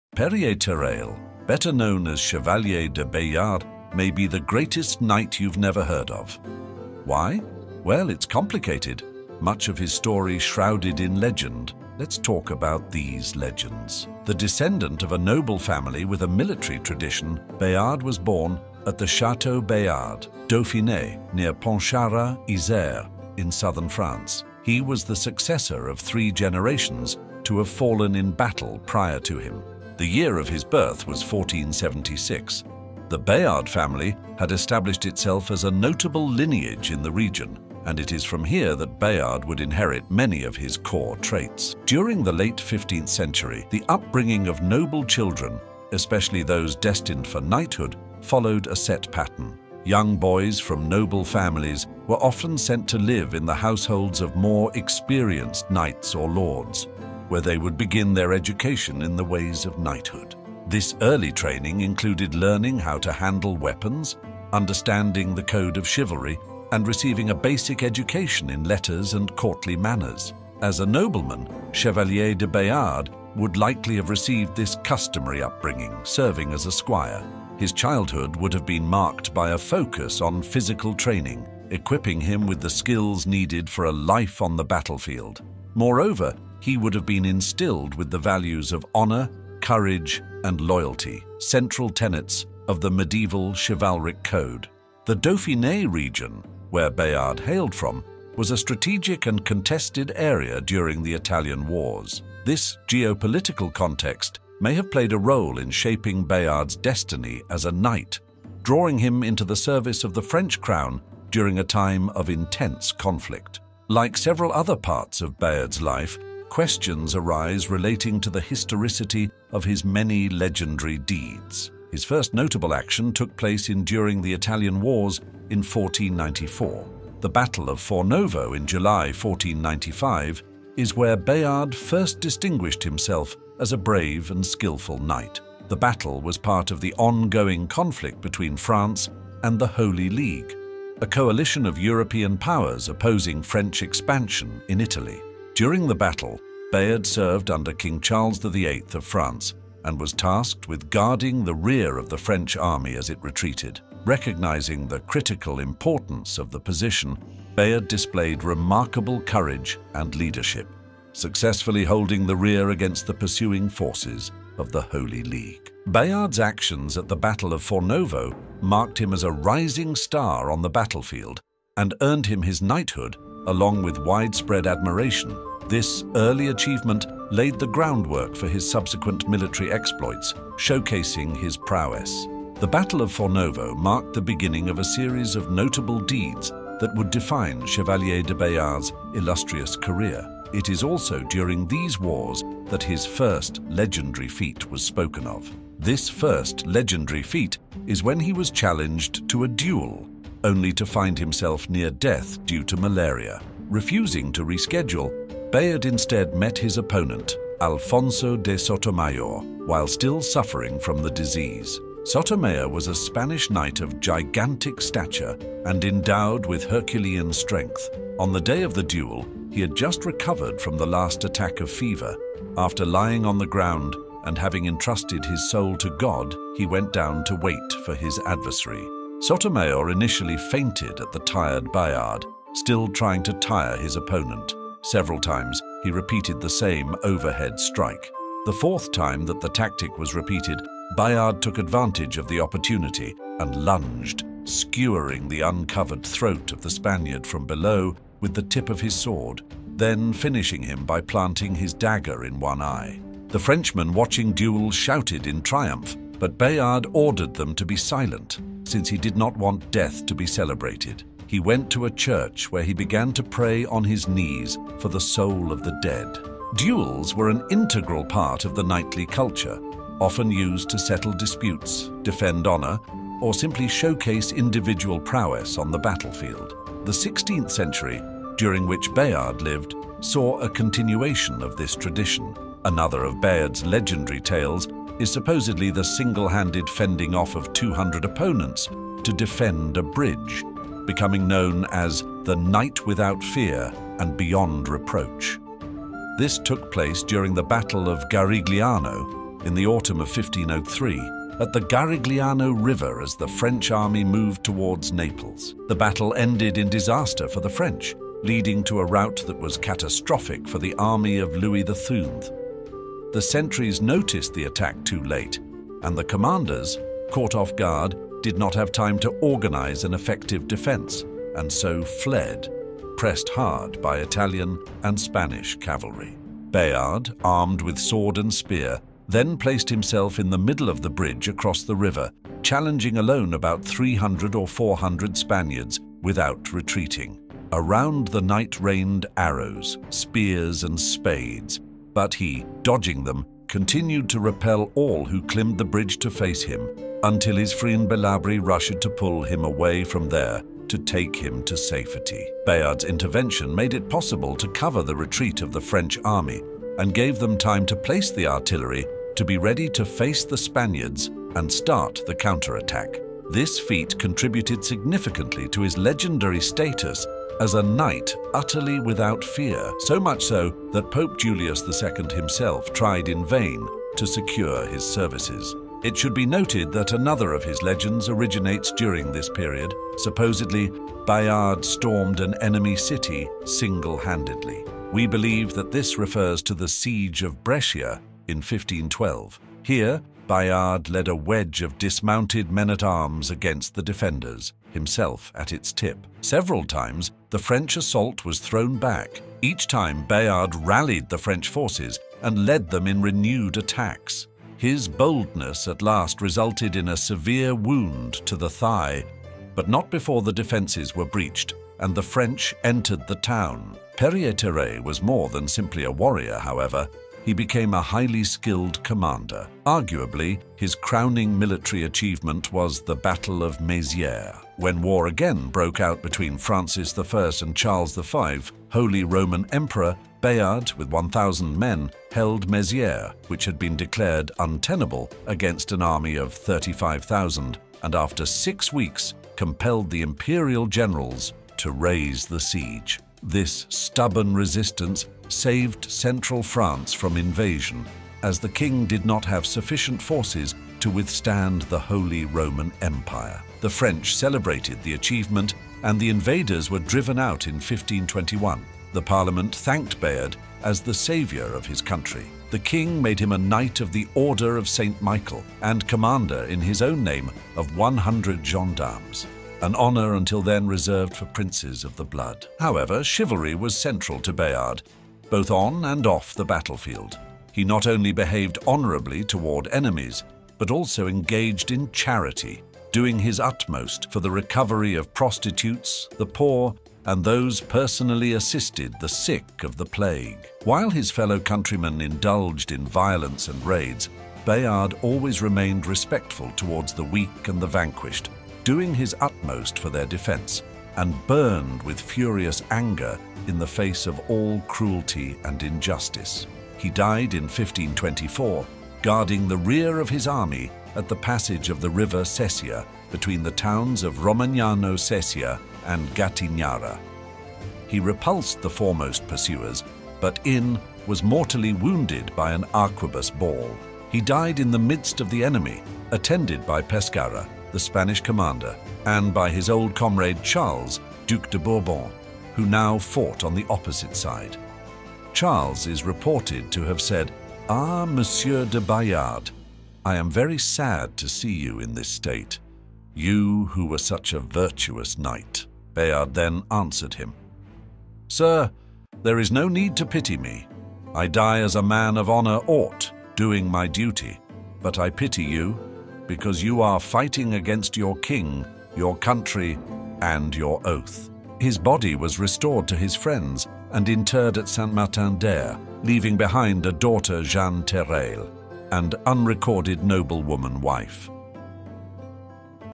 The GREATEST Knight You've Never Heard Of - Chevalier de Bayard
Perrier Terrail, better known as Chevalier de Bayard may be the greatest knight you’ve never heard of. Why? Well, it’s complicated, much of his story shrouded in legend – let’s talk about these legends.

The descendant of a noble family with a military tradition, Bayard was born at the Château Bayard, Dauphiné (near Pontcharra, Isère) in southern France. He was the successor of three generations to have fallen in battle prior to him.

The year of his birth was fourteen-seventy-six. The Bayard family had established itself as a notable lineage in the region, and it is from here that Bayard would inherit many of his core traits.

During the late 15th century, the upbringing of noble children, especially those destined for knighthood, followed a set pattern. Young boys from noble families were often sent to live in the households of more experienced knights or lords, where they would begin their education in the ways of knighthood.
This early training included learning how to handle weapons, understanding the code of chivalry, and receiving a basic education in letters and courtly manners. As a nobleman, Chevalier de Bayard would likely have received this customary upbringing, serving as a squire.
His childhood would have been marked by a focus on physical training, equipping him with the skills needed for a life on the battlefield. Moreover, he would have been instilled with the values of honor, courage, and loyalty, central tenets of the medieval chivalric code.